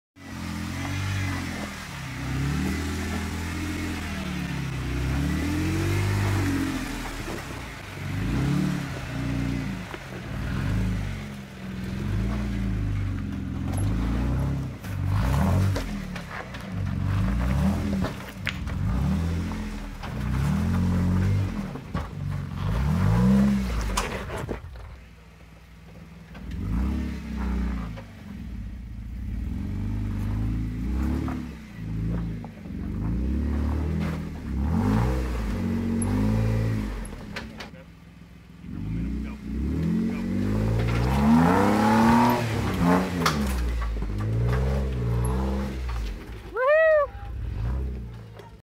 Rausch Creek Climb - Shoot the Moon
Krawlin 4 Cops 2017 ... Shake down run of the tons and 40s. Great day wheeling.